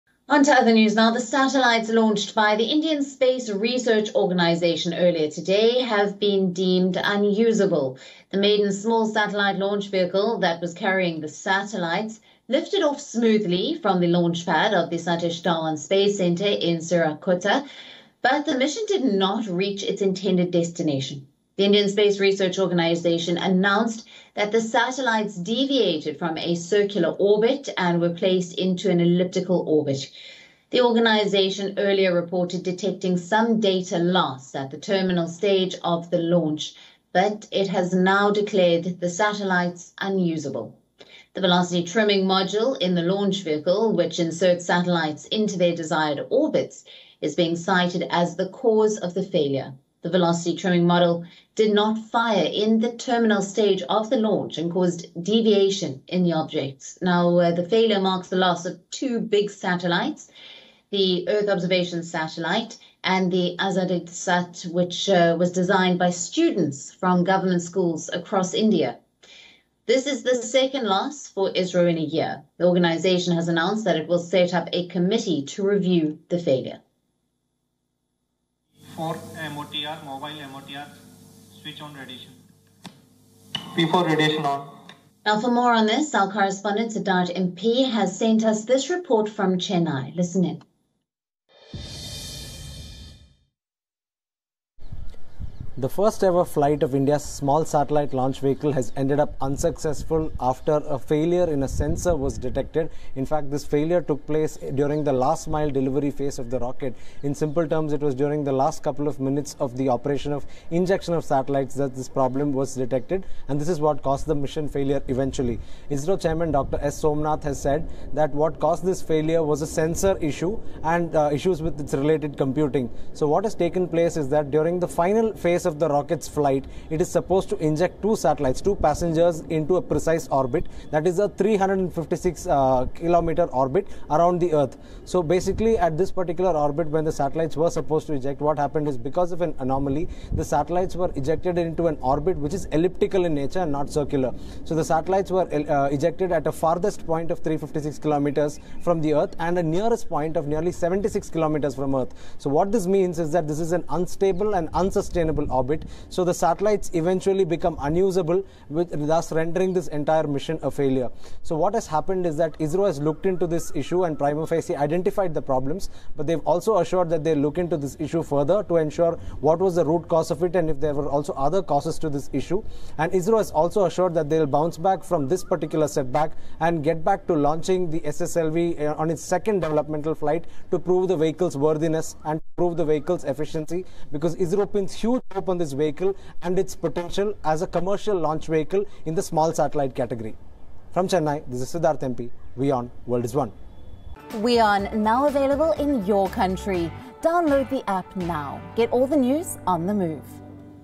India's newly launched satellites deviated from circular orbit | Latest World News | WION
The satellites launched by the Indian space research organization have been deemed unusable. The maiden small satellite launch vehicle that was carrying the satellites lifted off smoothly from the launch pad but the mission did not reach its intended destination.